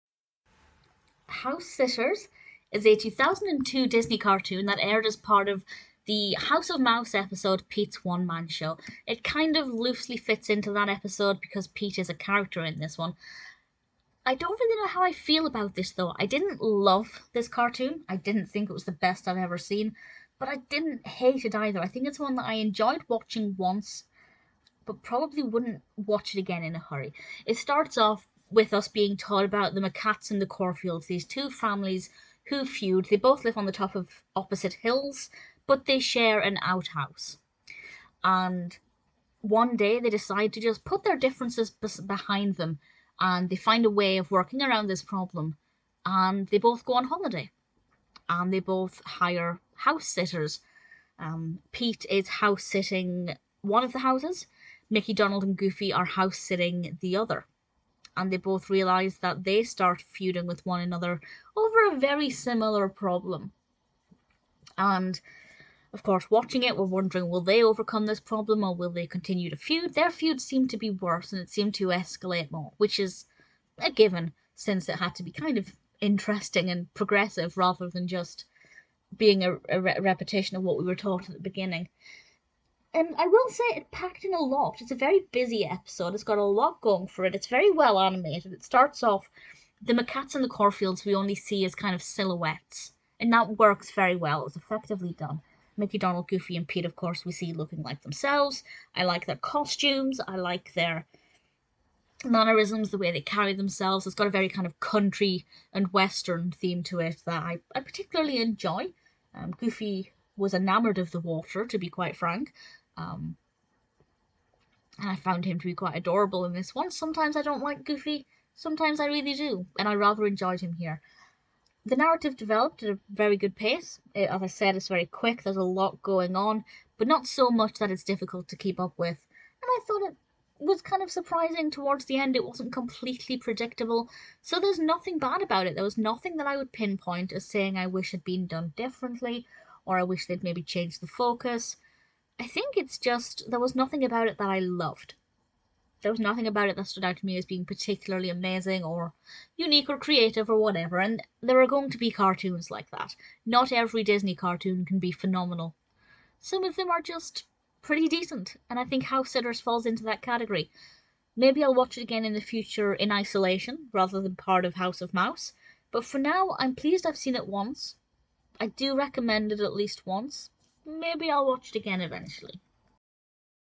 Housesitters 2002 Disney Mickey, Donald, and Goofy Film | Review
MY BOOKS What's your review of the 2002 Disney animated cartoon short film Housesitters? It features Mickey Mouse, Donald Duck, and Goofy. [OPEN FOR LINKS AND FACTS!] Housesitters DonaldDuck MickeyMouse Goofy Do you want to watch Housesitters? Did you enjoy the full movie or just parts of it? Scroll down for more information!

Amy McLean

About Housesitters 2002

Housesitters aired as part of Disney's House of Mouse.